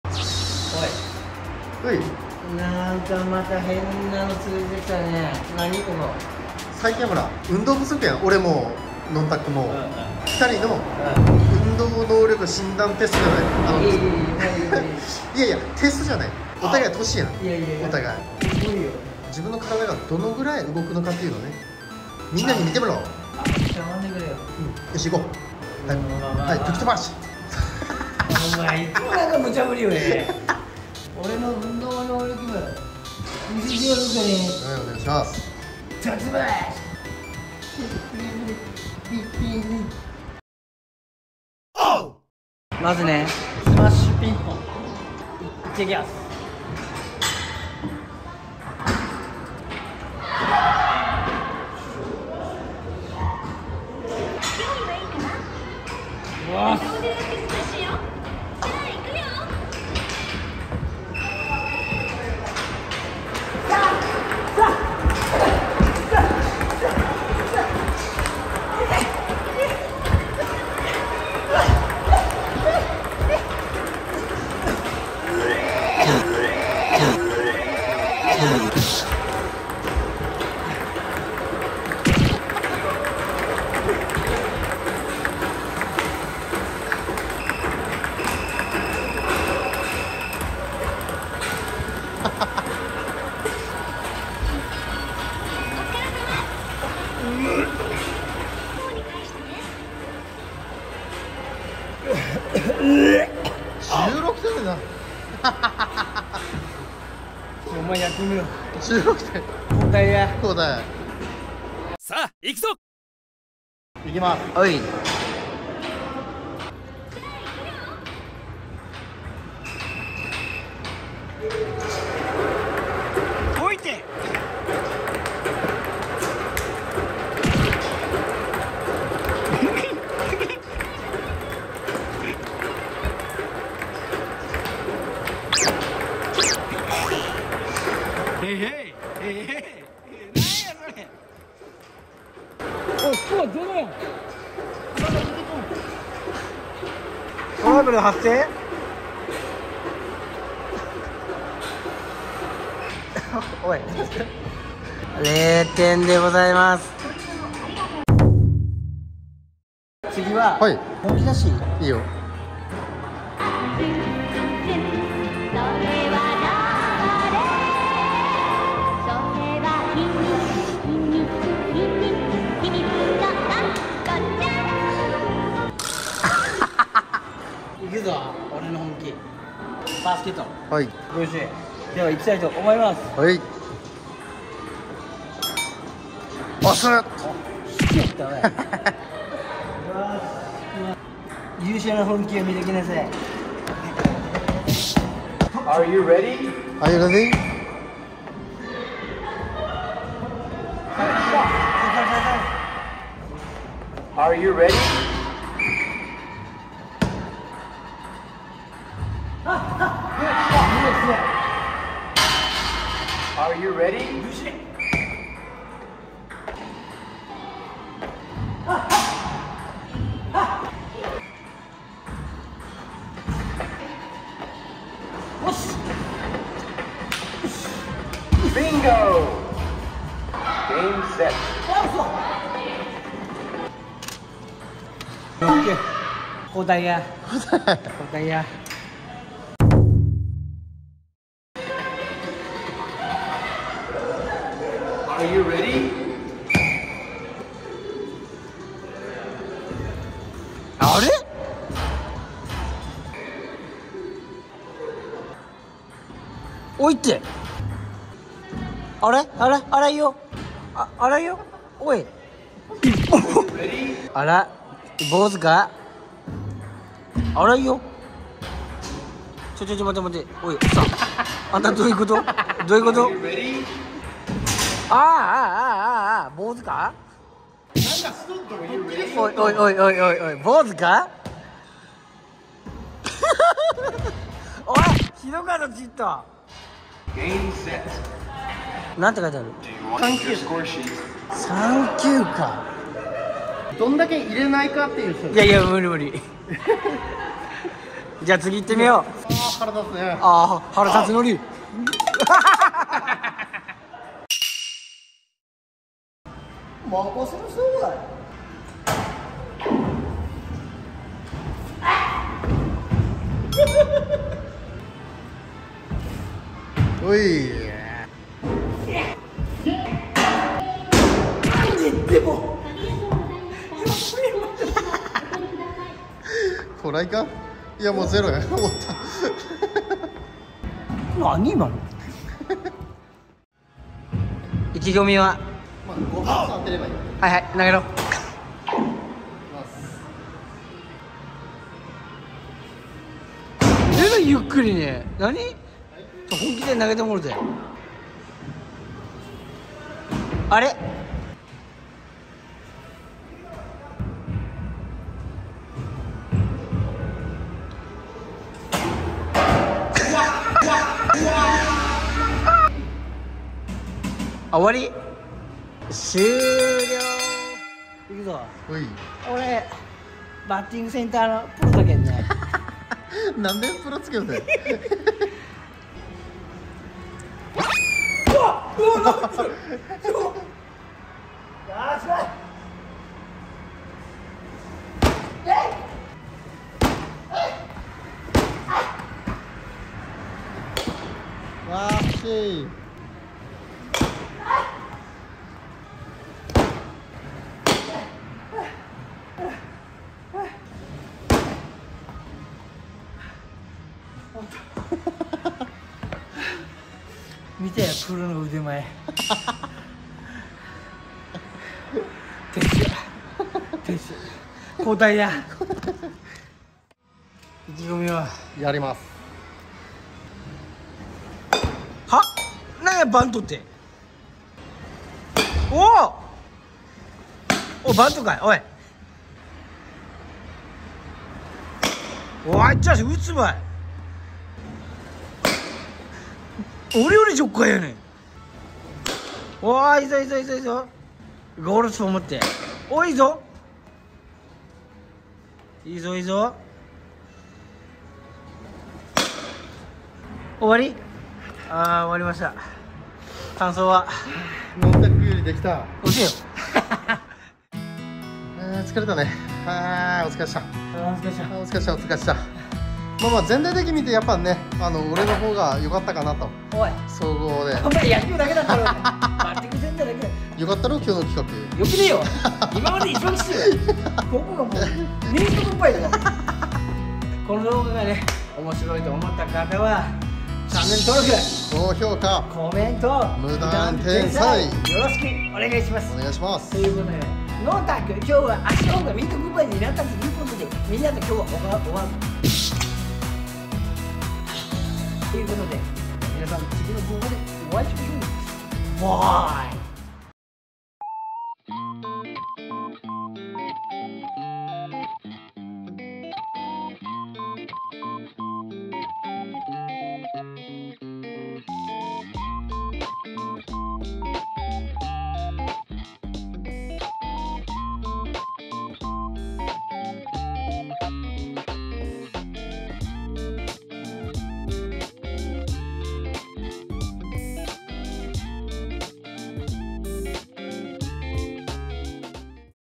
【運動測定】のんたっくvsサトシムはどっちが運動音痴なの？
勇者のんたっく【Instagram】

teamのんたっくドローン操縦士【Instagram】
Kumamoto Film